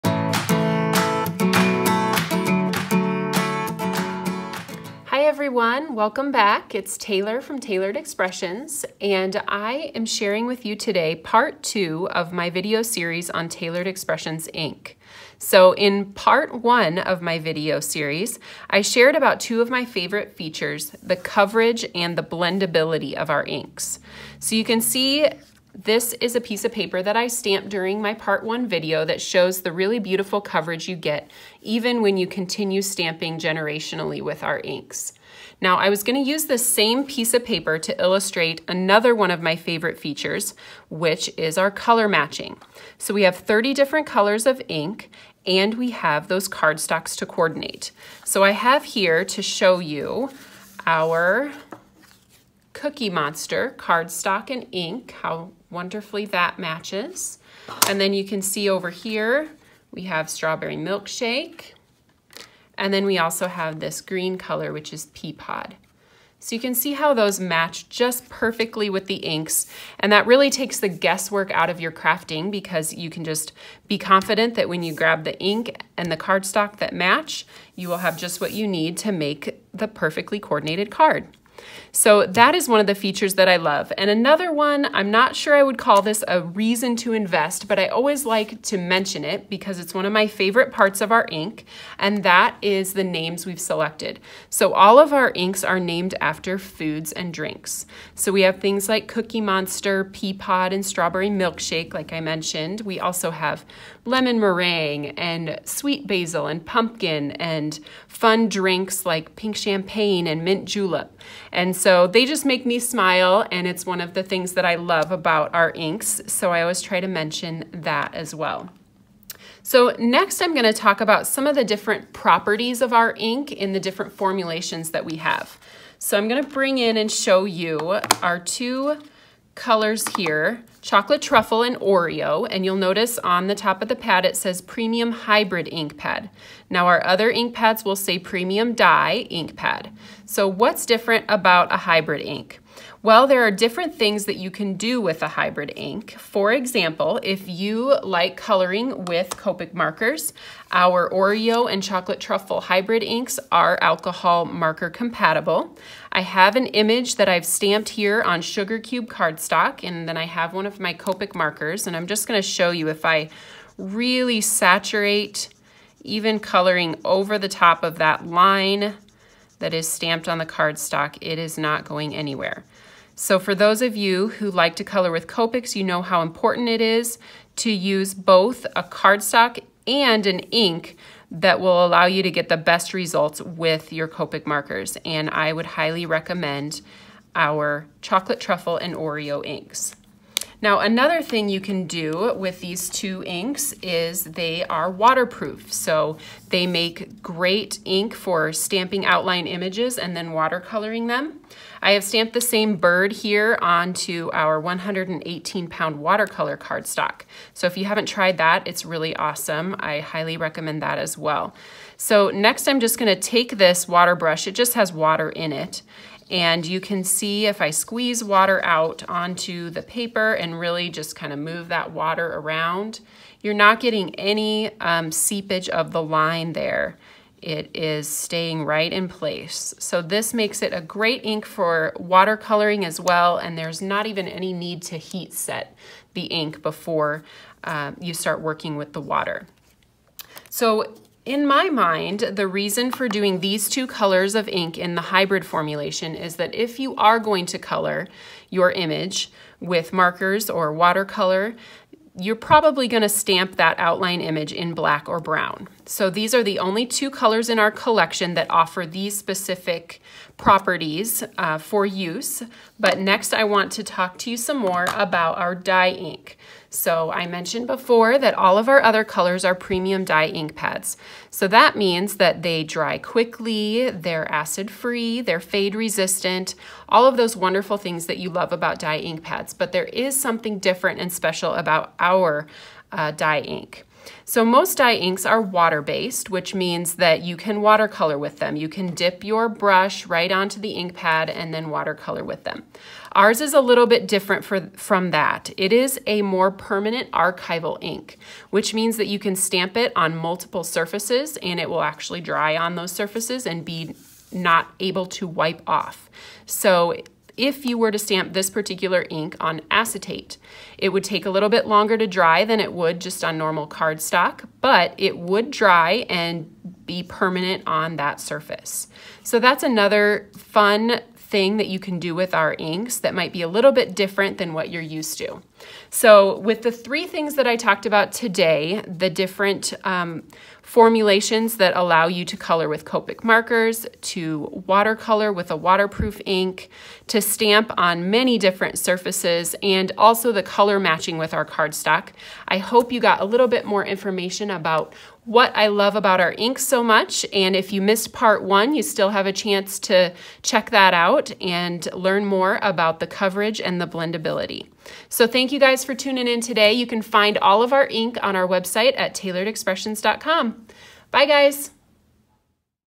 What is the Difference Between Dye and Hybrid Inks?
It's no secret there are a lot of inks on the market, but which one should you use for your handmade cards? We'll explain.

In this video, Taylor explains the key differences between Taylored Expressions ink and other inks on the market. Taylor also explores the differences between TE's premium hybrid and dye ink and the capabilities of each.